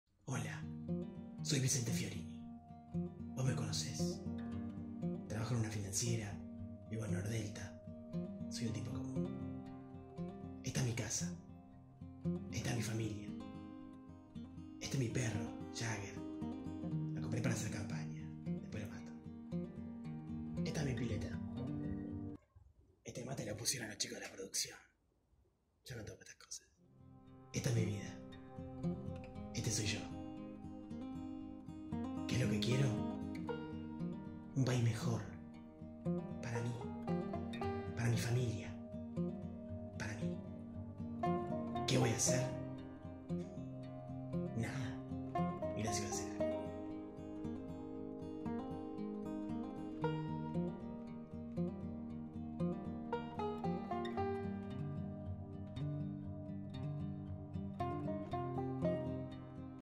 Campaña Electoral del Dr. Vicente Fiorini 2 - Argentina 2019
Dirección: Milton Ekman.

Vicente Fiorini es: Joaquín de las Carreras.

Sonido: Iara Ekman.